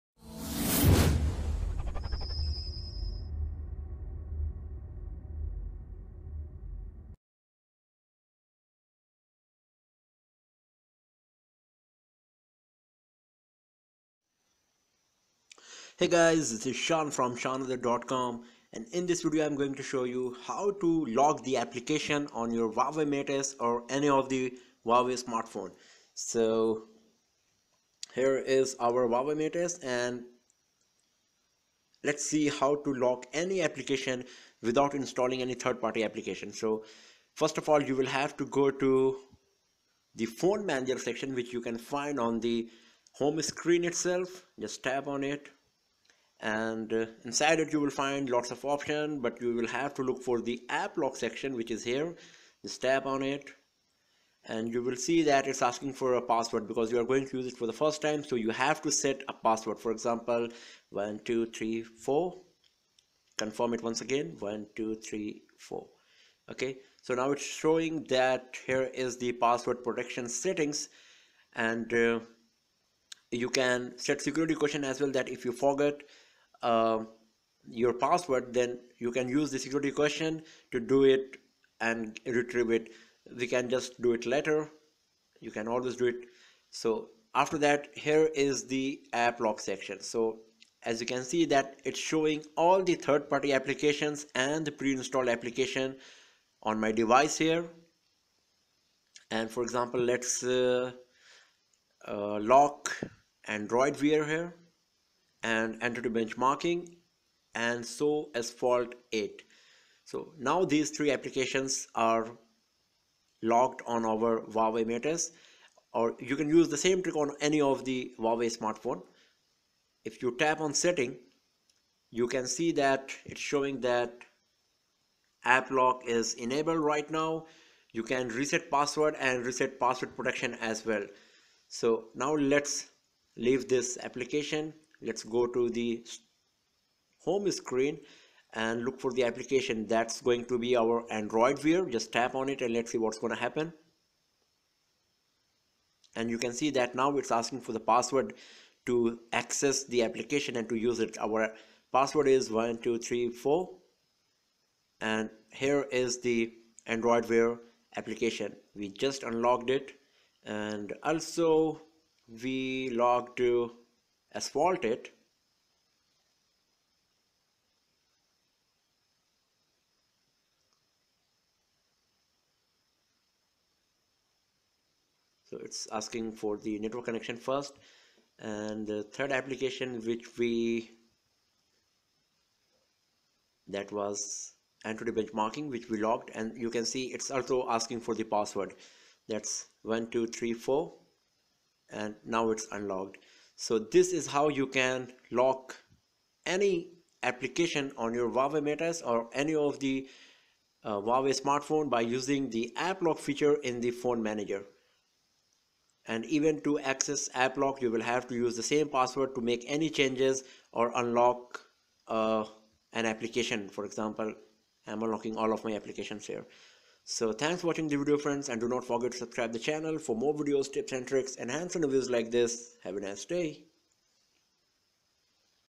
How to Lock Apps on Huawei Mate S, P8, Mate 7, Honor 7/6/6 Plus, G8 etc. Without Third-Party Apps
A quick tutorial about how to lock apps and games on Huawei smartphones like Huawei P8, Mate S, Ascend Mate 7, G8, Honor 7, Honor 6 Plus etc.

How to Hide Apps on Huawei P8, Mate S, Ascend Mate 7, G8, Honor 7, Honor 6 etc